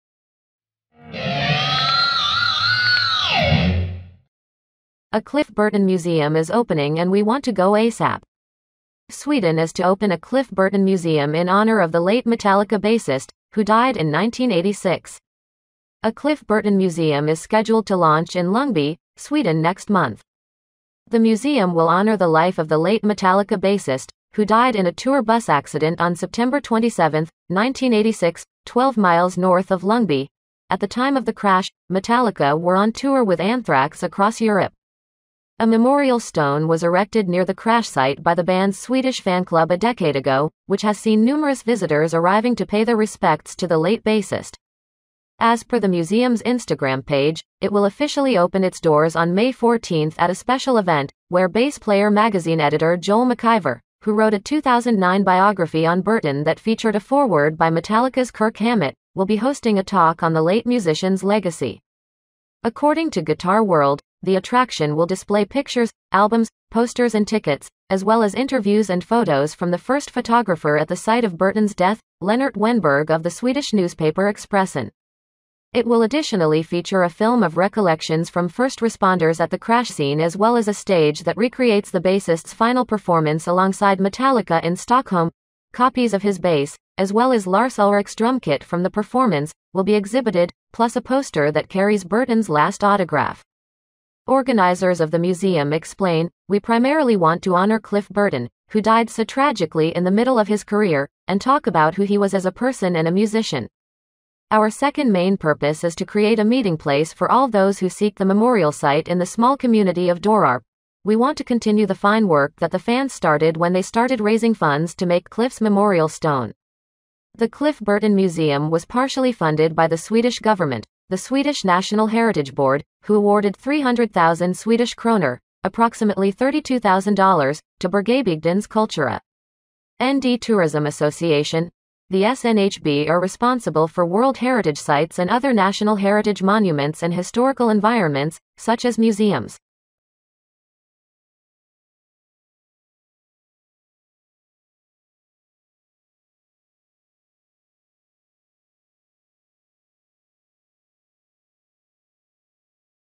Cliff Burton museum is opening
Sweden is to open a Cliff Burton museum in honour of the late Metallica bassist, who died in 1986